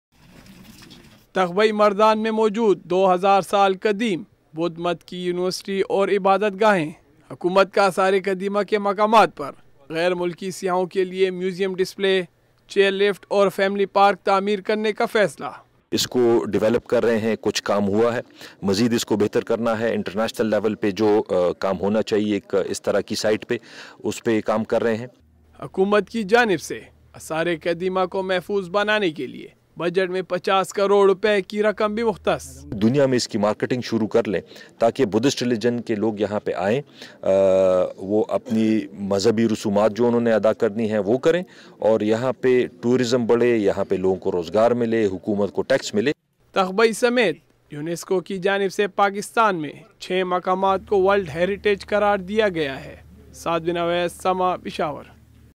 ATIF KHAN AND IFTIKHAR ALI MASHWANI VISIT TO TAKHT BAHI KANDARAT 3/1/2019
ATIF KHAN AND IFTIKHAR ALI MASHWANI VISIT TO TAKHT BAHI KANDARAT..SAMAA NEWS TV CHANNEL REPORT3/1/2019